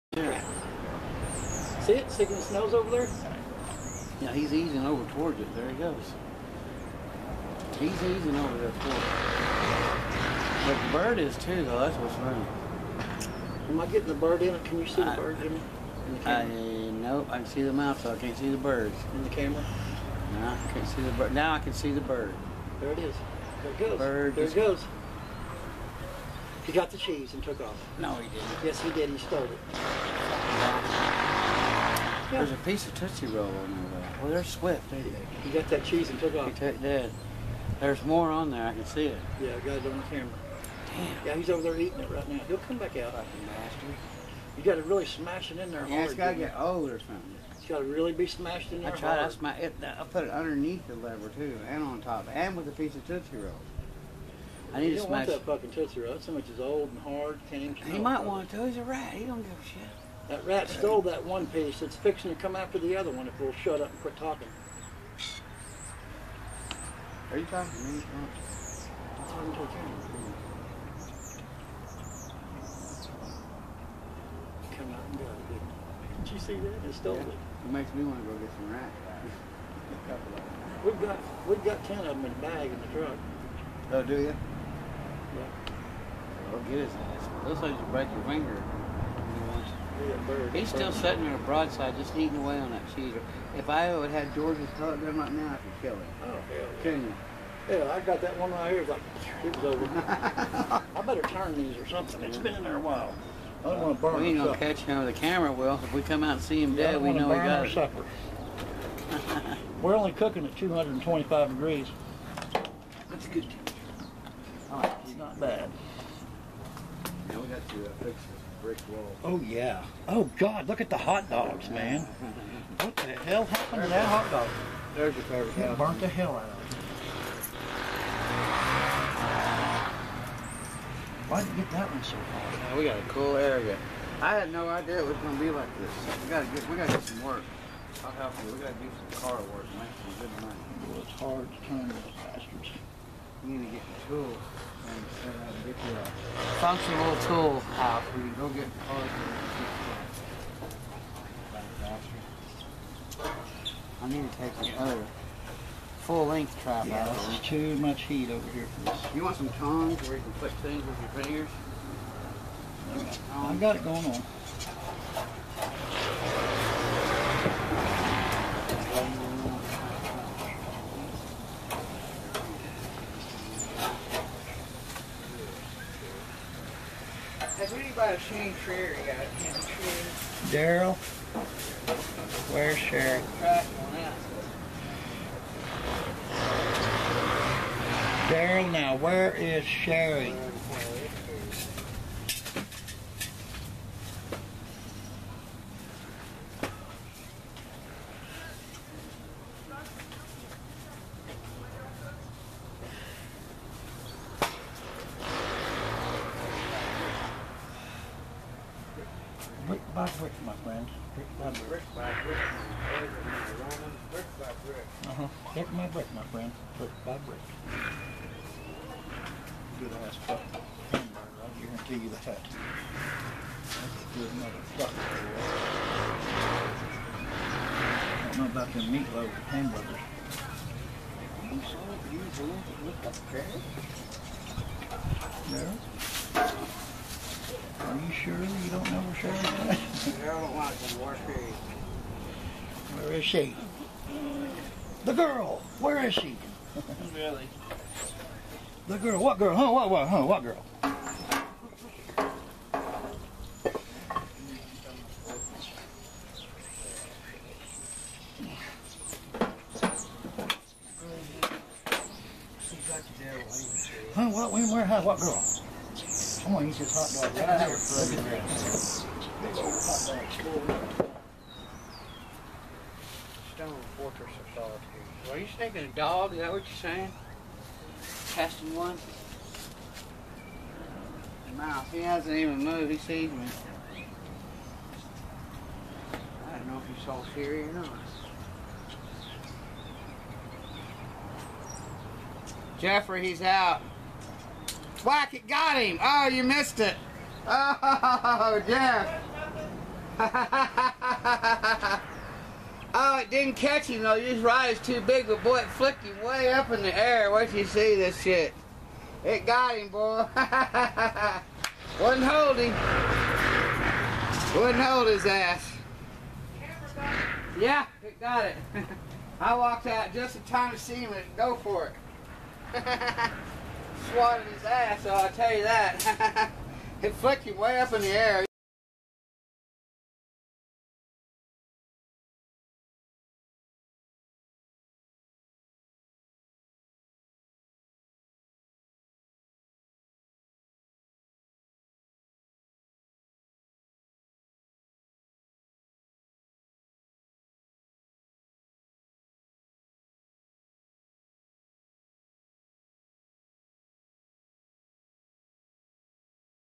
Catching rat with mouse trap!
We was cooking out one evening and this rat kept sticking his head out,  This is what happened.